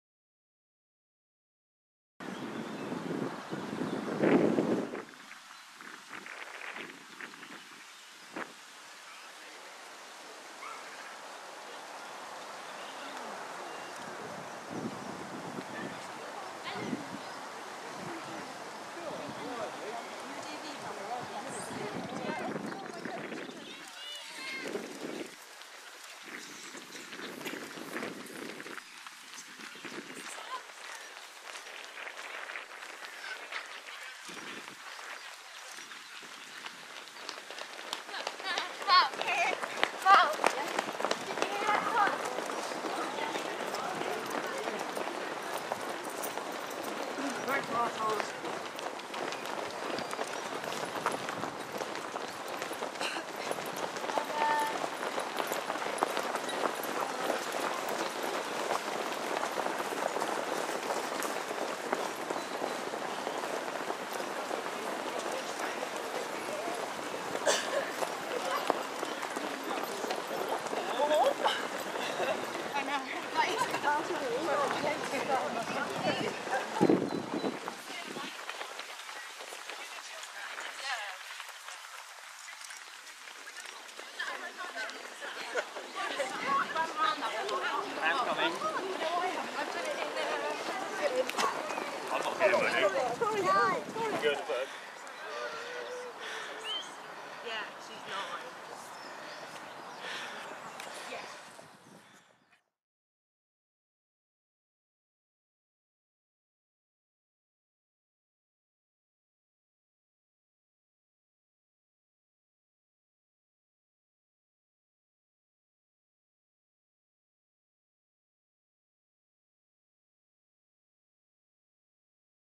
Stratford upon Avon parkrun number 4: the start
Almost the entire course of Stratford-upon-Avon parkrun was under water a week before this video was shot. The ground dried nicely and proved well able to take the weight of 173 runners and walkers, plus 23 volunteers. Stratford parkrun is a weekly 5km run (but you're allowed to walk). There's no entry fee: just turn up. you'll have all your times recorded.